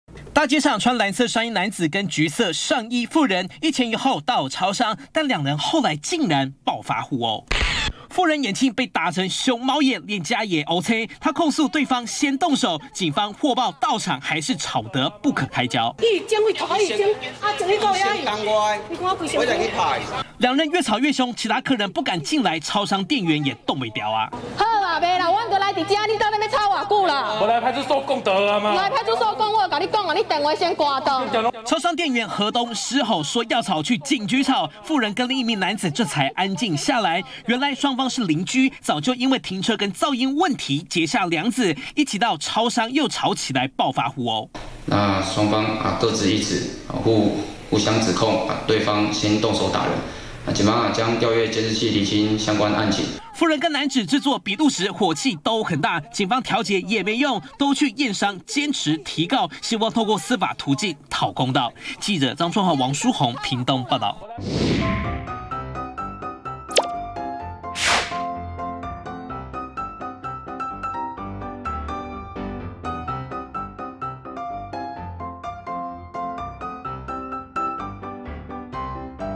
【每日必看】超商爆全武行! 婦人控鄰居尾隨動粗@中天新聞CtiNews 20210320
屏東東港一間超商裡，一名婦人跟鄰居爆發口角後互毆，婦人被打到眼睛腫成熊貓眼，臉頰也有黑青，事後PO文控訴，被鄰居尾隨毆打，但鄰居反控是婦人先動手，才會回擊，原來他們之前就疑似因為停車等等問題，結下樑子，警方試圖調解，但雙方各執一詞，堅持提告傷害。